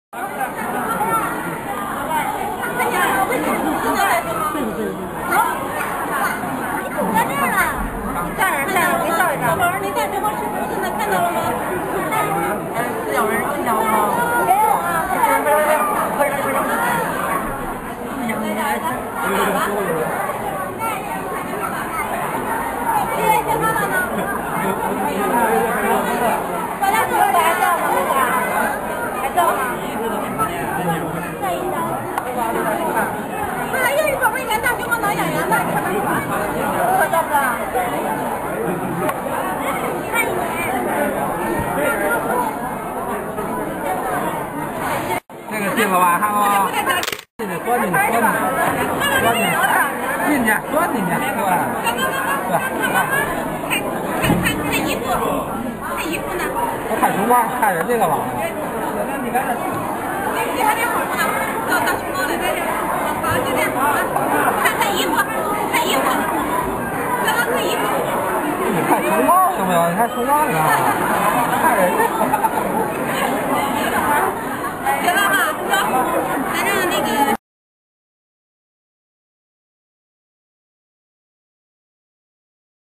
Male Panda Jing Hui (Er Bao) in Beijing Zoo 北京動物園大熊貓京惠（二寶）
Love youtube. Damn CCP & GFW. 譚作人，你媽媽喊你回家吃飯。 聲援調查512地震豆腐渣學校的義士譚作人，抗議匪共政府陷害譚作人。